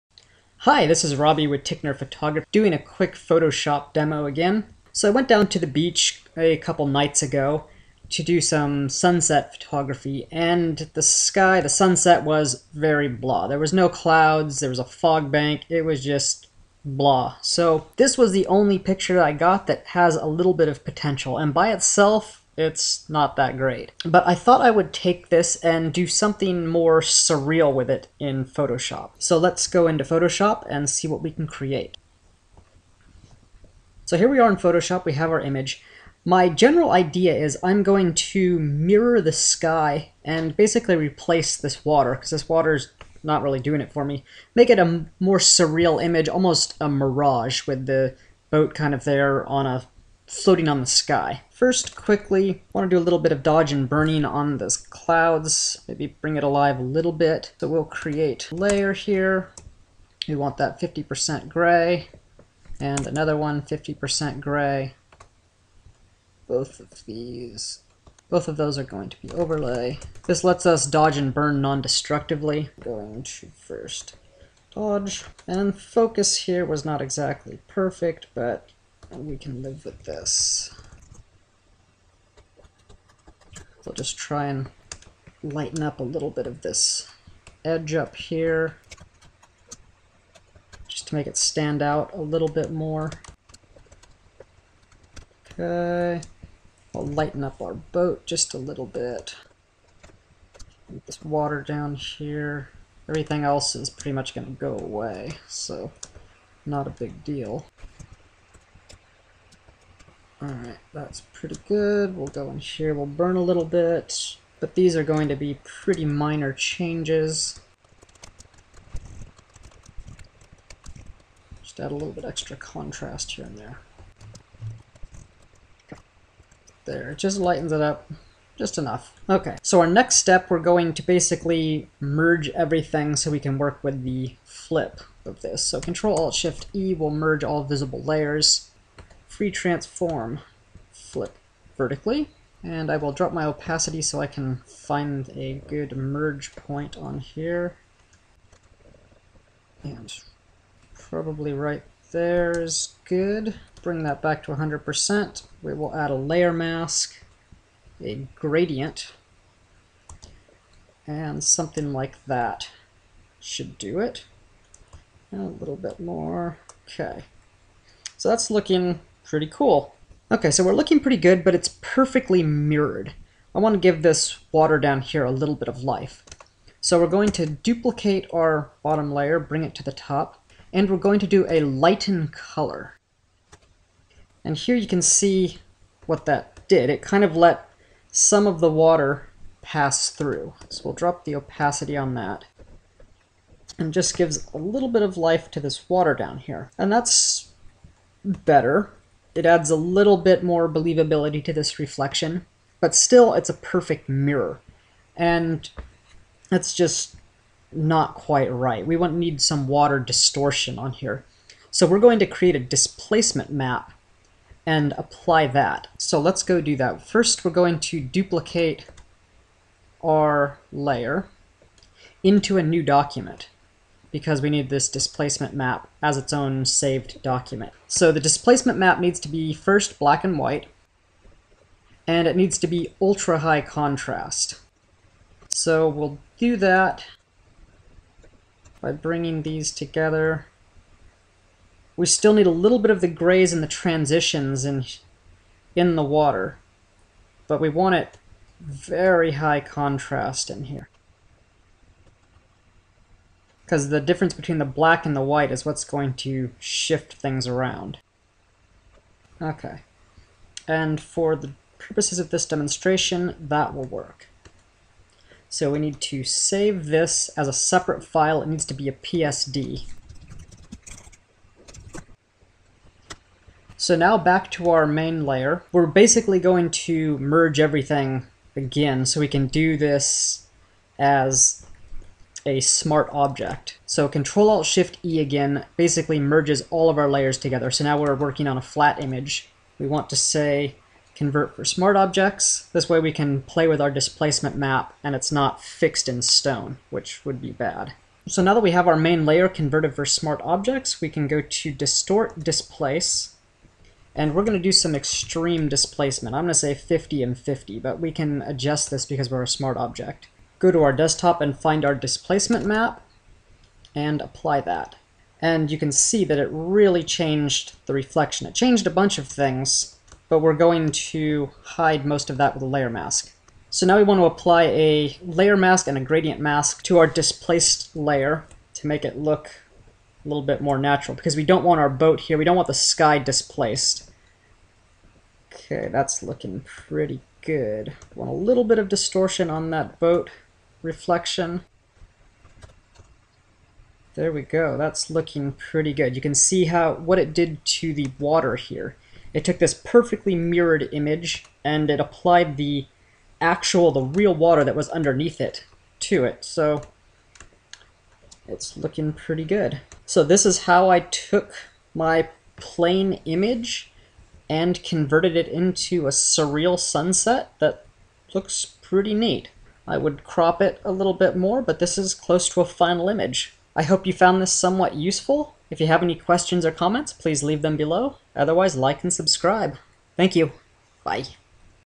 Surreal Sunset Photoshop Tutorial - Boat Mirage, floating on clouds
In this  Tutorial, I take a plane image I shot with my Sony Alpha a77 at Ocean Beach in San Diego, and transform it with photoshop in to a surreal sunset of a boat floating on the sky.

Link to Adobe Photoshop CS6

Adobe Lightroom 4